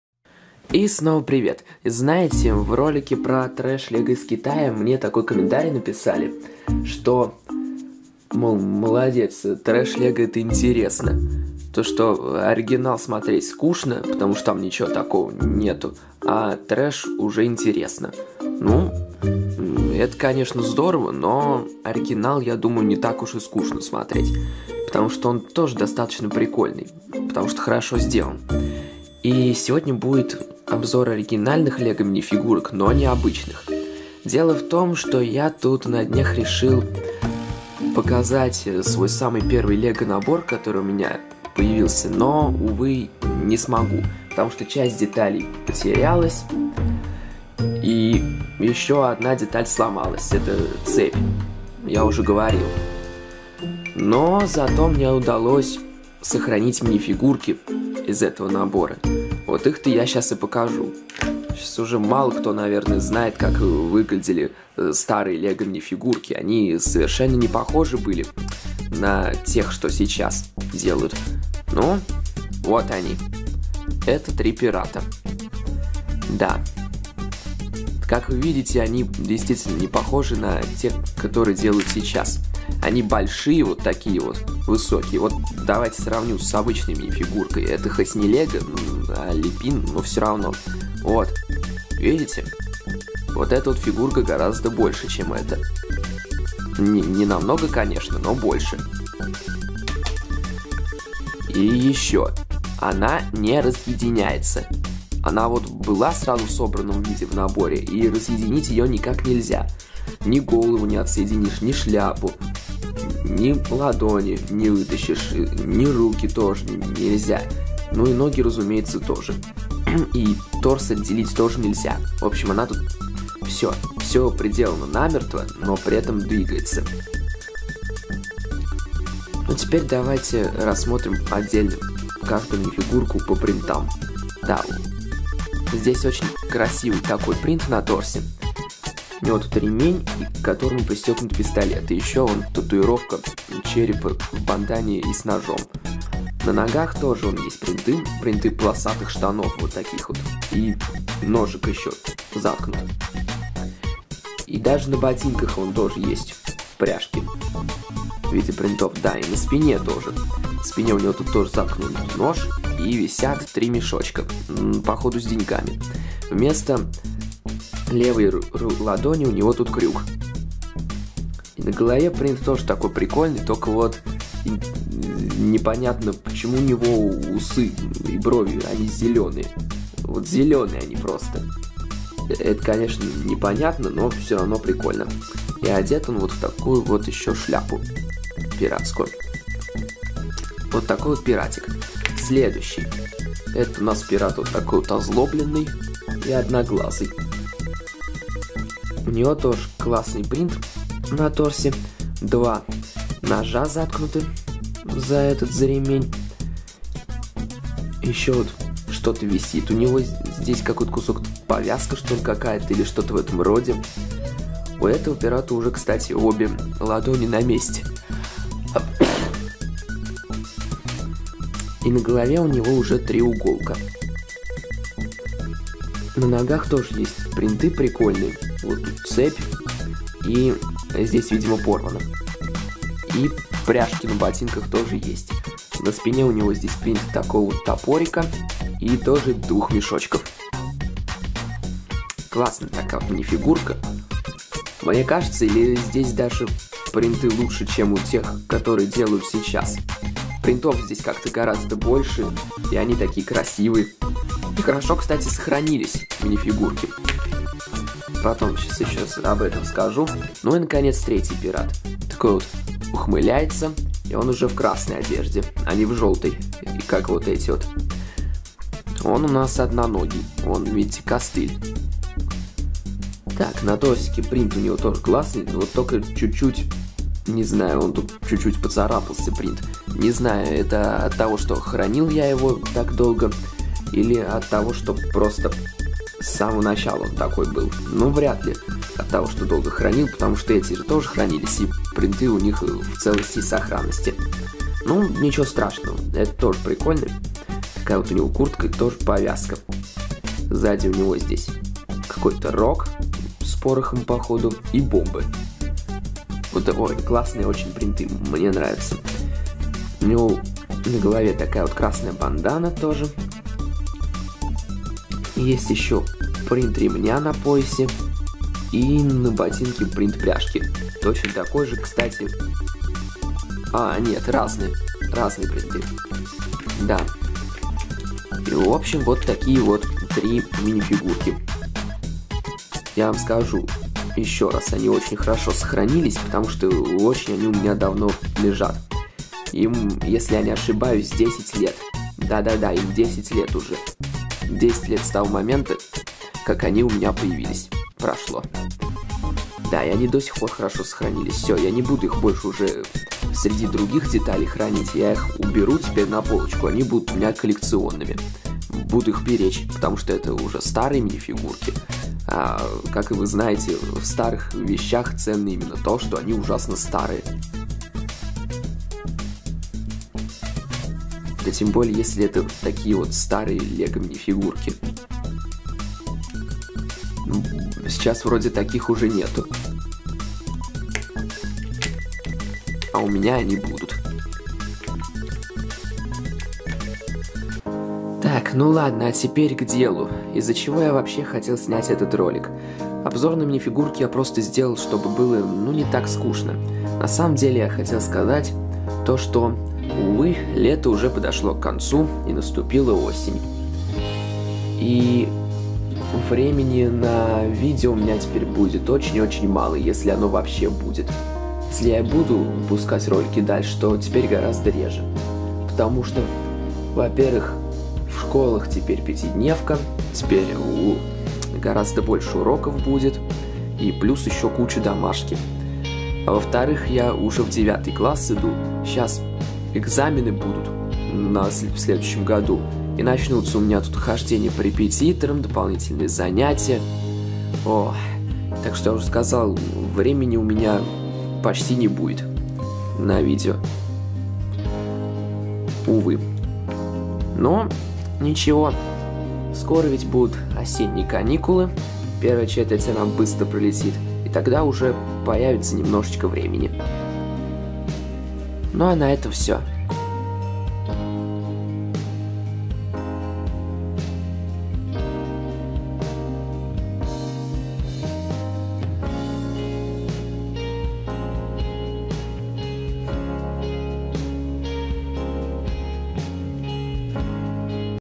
Мои самые первые LEGO минифигурки! Грустный конец лета
Обзор моих самых первых LEGO минифигурок и не очень хорошие новости. Да, да, началась учёба, и времени на видео теперь меньше.